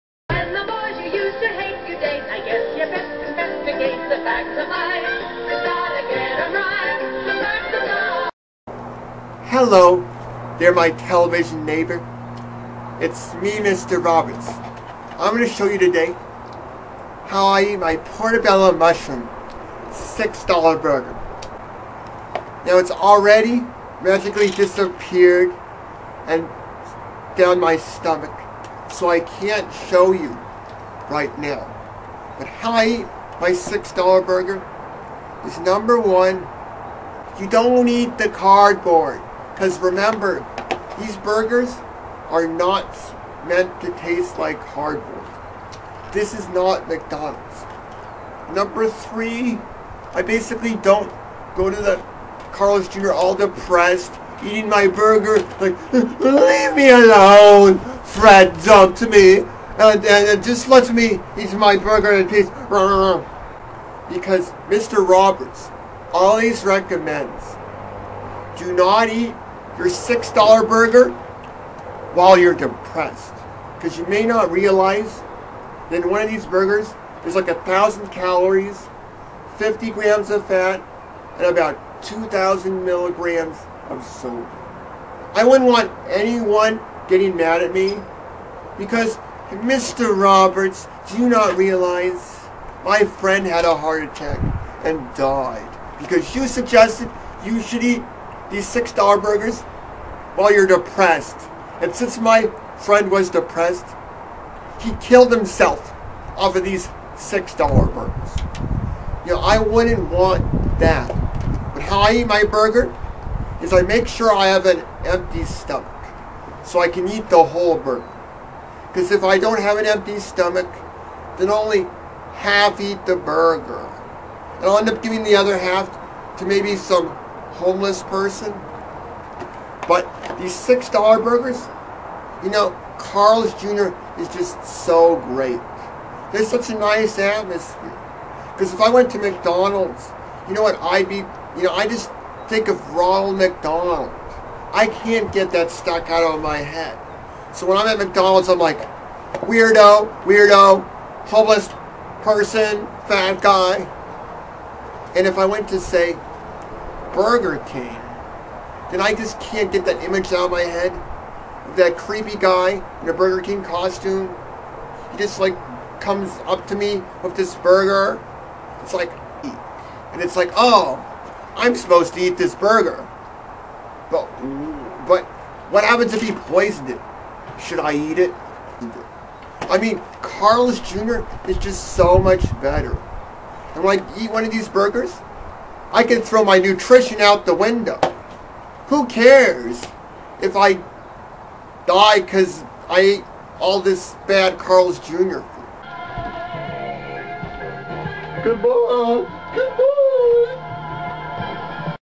Re:JOHN'S PUNISHMENT! Eating Carl's Jr John's Way
How Mr. Roberts Eats his Carls Jr. 6 Dollar Burger.